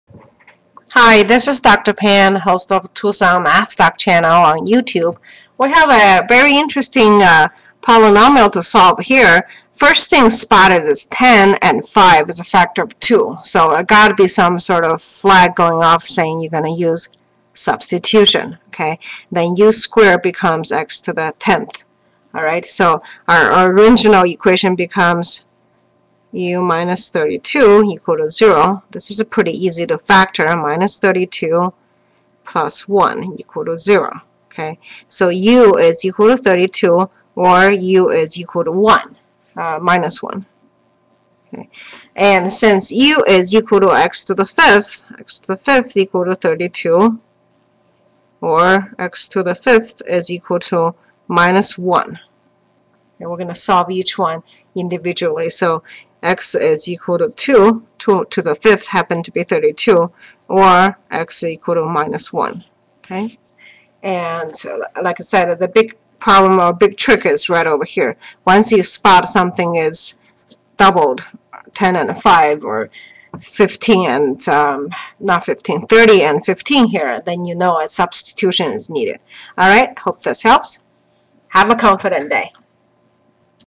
Solve Higher Order Polynomial Equation: x^10-31x^5-32=0
A production. Dr. Pan makes learning math Fun.
Solve the equation x^10-31x^5-32=0